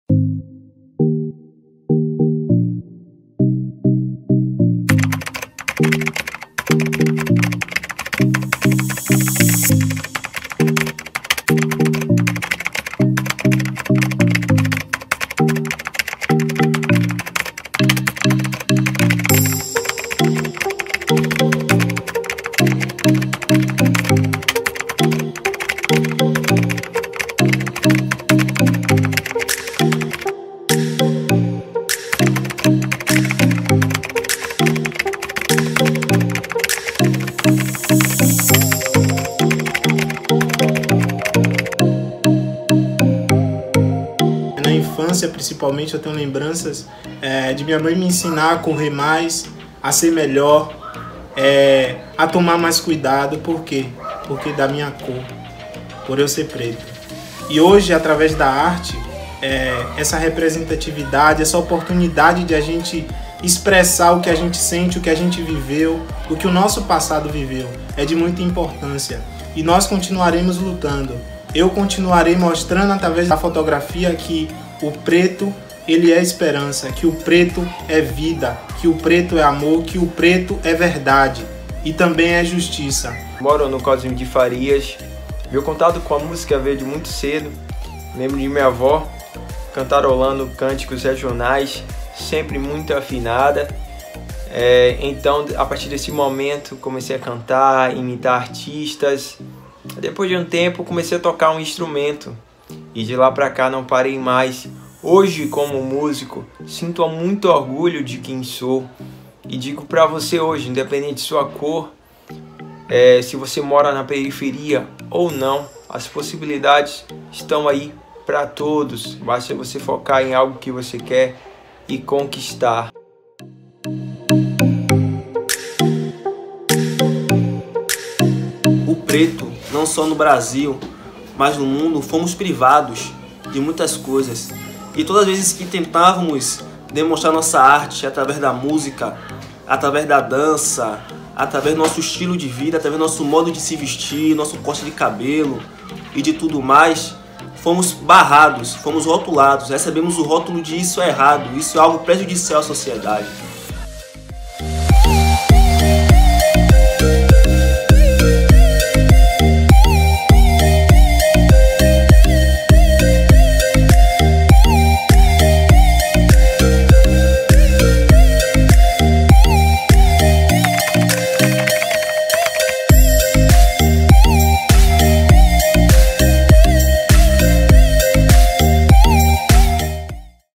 Moda e representatividade
A moda comunica muito mais que tendências, lançamentos e produtos de
desejo. Através das imagens que cria, possui a força de inspirar, influenciar, evidenciar e representar relevantes questões sociais como a luta antirracista. Esse talvez seja seu papel mais importante, o da representatividade. Como escreveu o intelectual e presidente da Fundação Luiz Gama, professor Silvio Luis de Almeida, em sua obra O Racismo Estrutural, a importância da representatividade está em “desmantelar as narrativas discriminatórias que sempre colocam minorias em locais de subalternidade”.

Pensando em todas essas questões que fervilham no mundo, decidimos convidar um jovem fotógrafo negro baiano, Renan Benedito, 23 anos (@renabenedit) - que, com pouco tempo de atuação, já soma trabalhos para Renner, Itaú e exposições no Museu de Arte Moderna da Bahia -, para que criasse imagens relacionando a moda com a representatividade negra. Desafio fácil para um fotógrafo autodidata que se considera um apaixonado por contar histórias através da fotografia.

Foi o que ele fez neste ensaio, protagonizado por outros dois jovens negros, de bairros periféricos de Salvador, que já enfrentaram situações de preconceito pela cor da pele. “Continuarei mostrando através da fotografia que o preto é esperança, vida, amor e verdade” sentencia Renan.

Confira o resultado desse trabalho que exalta verdadeiras expressões artísticas e lança uma mensagem de esperança. E conheça também as histórias dos modelos. O editorial ainda teve a arte criada pelo também jovem artista visual negro Igor Aquino (@astronautade.marmore).

Reportagem e produção: @paulamagalhaes1 e @leo_amaral